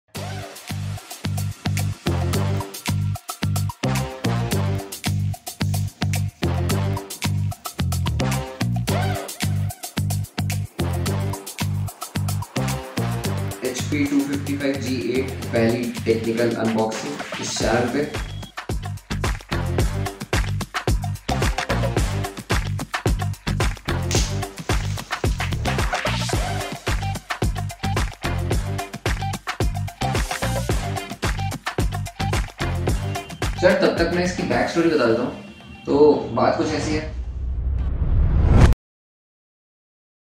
Unboxing new Laptop | HP 255 G8 Ryzen 5 | Ayush Agarwal
In this particular video, I am unboxing my new laptop that I got from college as a prize for certain criteria (explained in full video, link below).